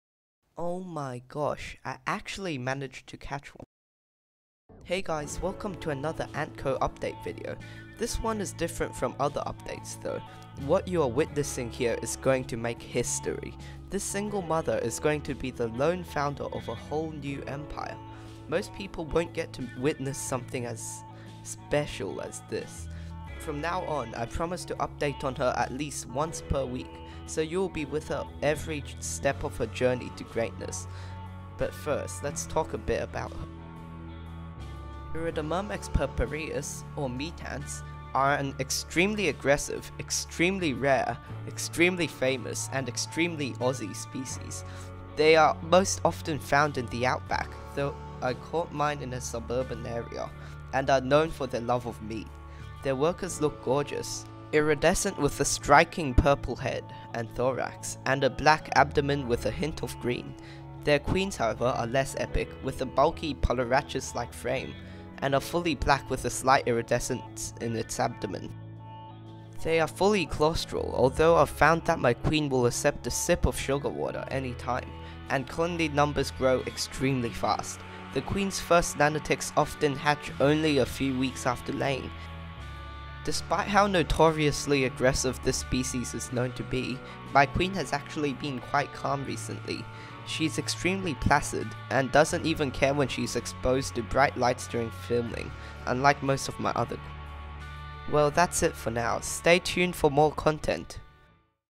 *NEW QUEEN* Iridomyrmex Purpureus Queen: Genesis |Presented & Narrated By Ant Co
Hey guys, welcome to another Ant Co update video! This one is different from other updates though… What you are witnessing here is going to make history. This single mother is going to be the lone founder of a whole new empire! Most people won’t get to witness something as special as this! From now on, I promise to update on her at least once per week, so you will be with her every step of her journey to greatness!